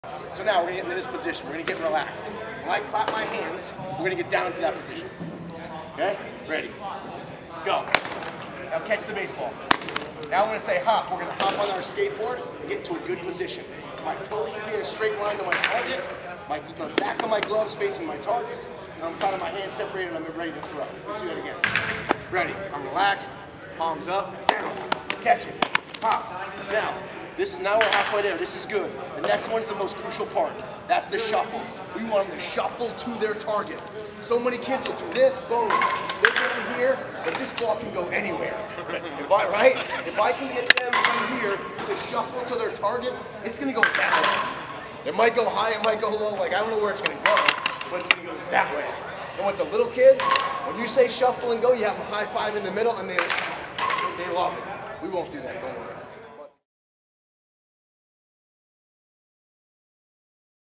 Duke Baxter is demonstrating to youth coaches how to teach fielding a groundball
Duke Baxter is teaching youth coaches how to teach youth baseball players to field a groundball and then how to activate the lower half to get momentum into their throw.

►Where to follow and listen to Zoned and Duke Baxter